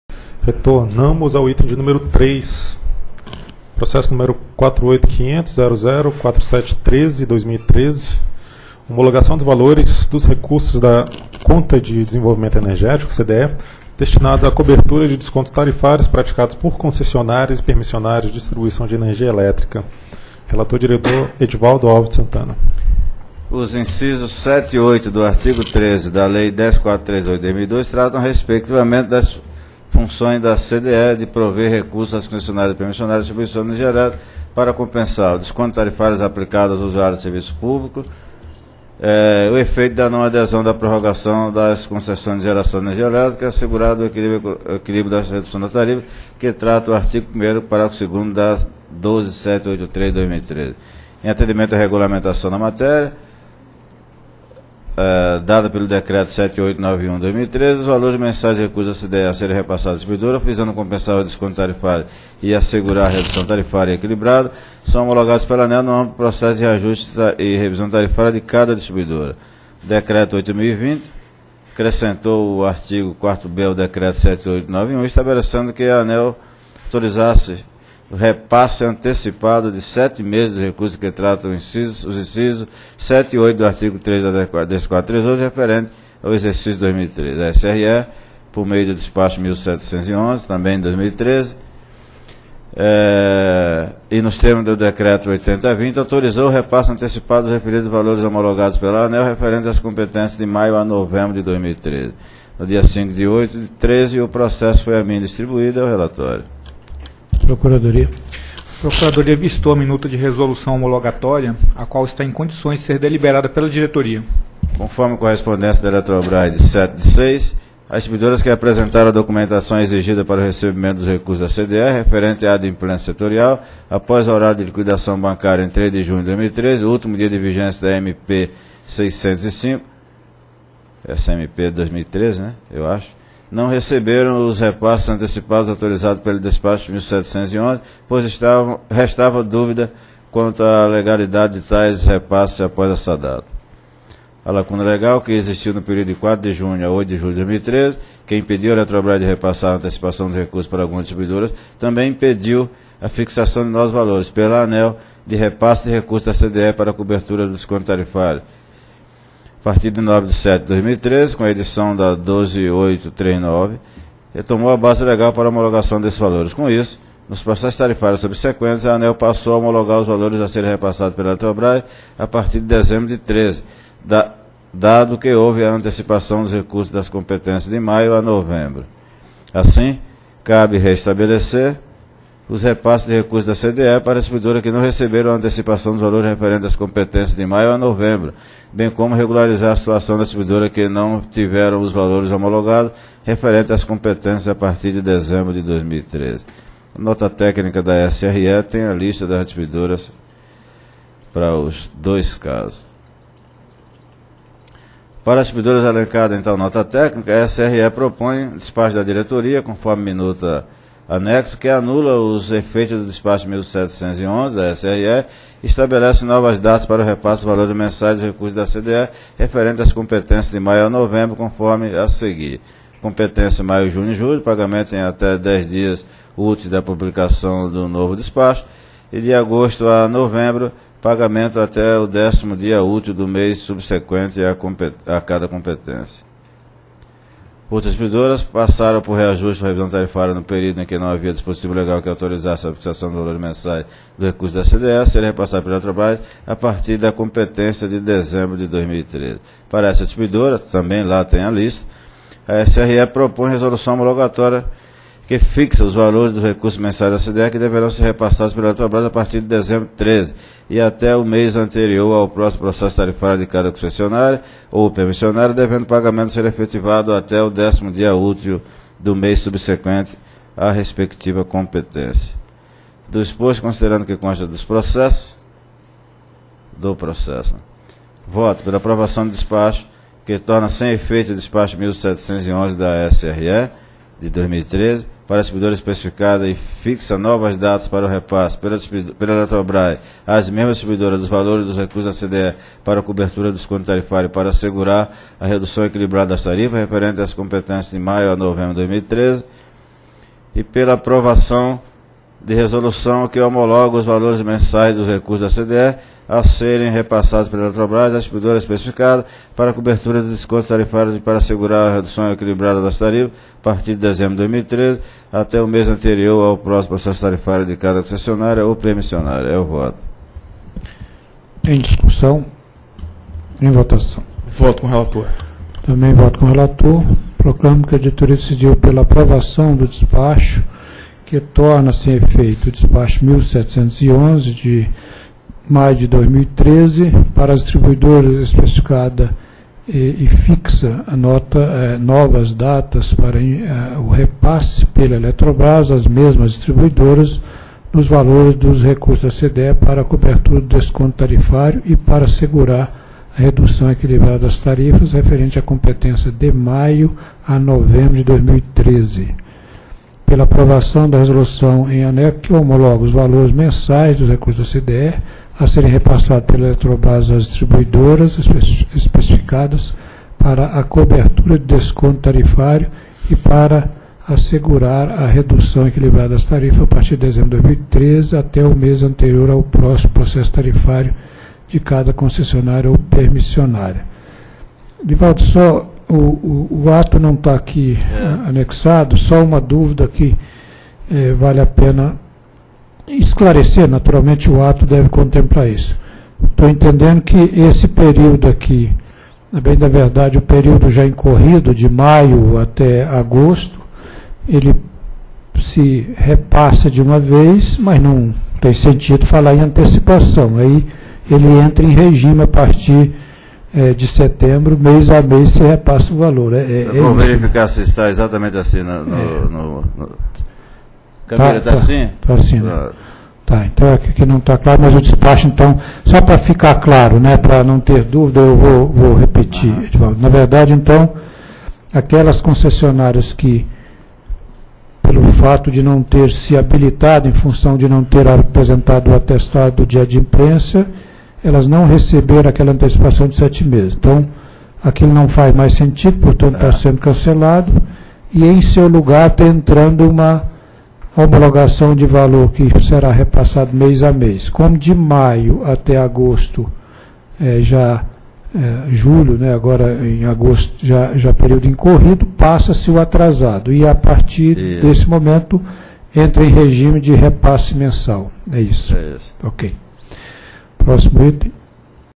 Item 3 - 30ª Reunião Pública Ordinária ANEEL 13/08/2013 61
Assunto: Homologação dos valores dos recursos da Conta de Desenvolvimento Energético -- CDE destinados à cobertura de descontos tarifários praticados por concessionárias e permissionárias de distribuição de energia elétrica. Área Responsável: Superintendência de Regulação Econômica -- SRE.